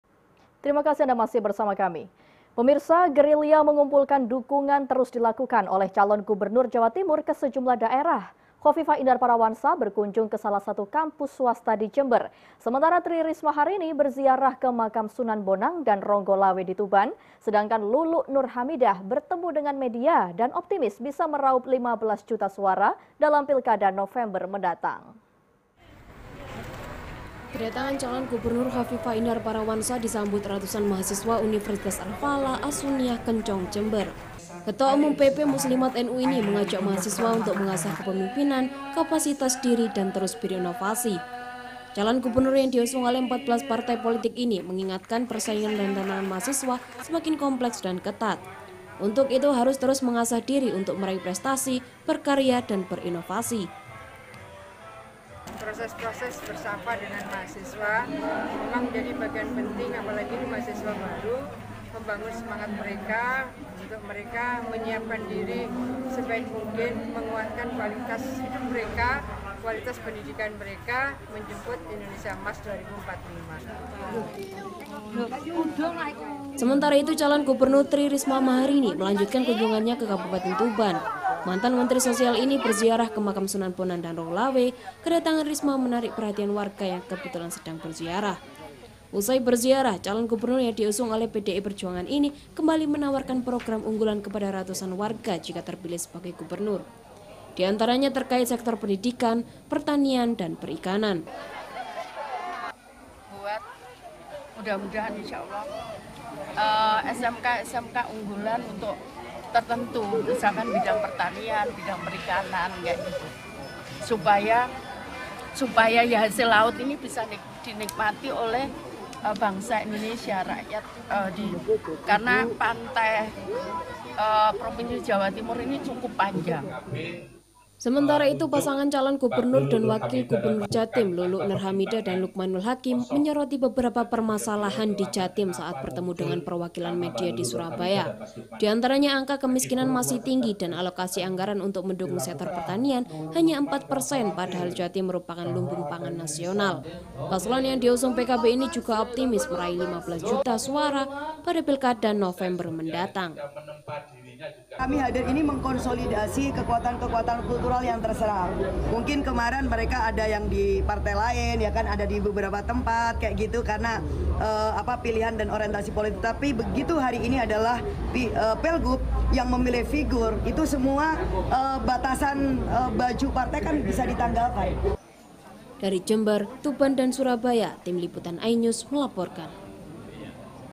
KHOFIFAH AJAK MAHASISWA BERINOVASI JAWAB PERSAINGAN GLOBAL
GERILYA MENGUMPULKAN DUKUNGAN TERUS DILAKUKAN OLEH CALON GUBERNUR JAWA TIMUR KE SEJUMLAH DAERAH. KHOFIFAH INDAR PARAWANSA, BERKUNJUNG KE SALAH SATU KAMPUS SWASTA DI JEMBER. SEMENTARA TRI RISMAHARINI BERZIARAH KE MAKAM SUNAN BONANG DAN RONGGOLAWE DI TUBAN. SEDANGKAN LULUK NUR HAMIDAH BERTEMU DENGAN MEDIA DAN OPTIMIS BISA MERAUP 15 JUTA SUARA DALAM PILKADA NOVEMBER MENDATANG.